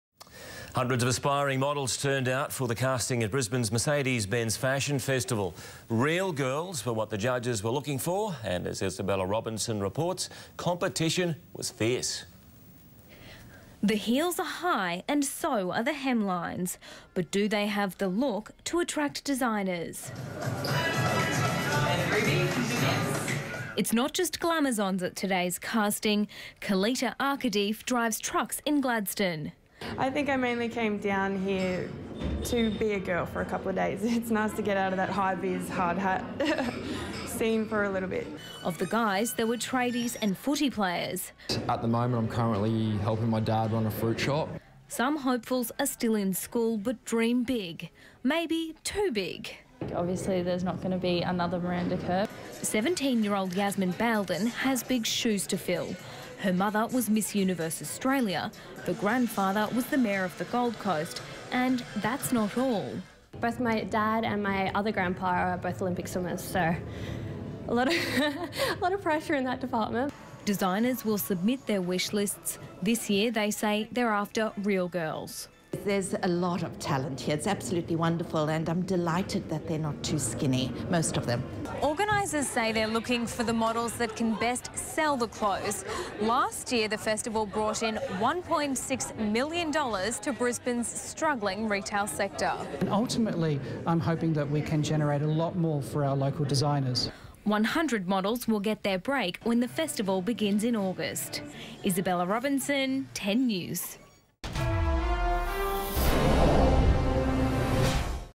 Model search
Hundreds of aspiring models turned out for the casting of Brisbane's Mercedes Benz Fashion Festival.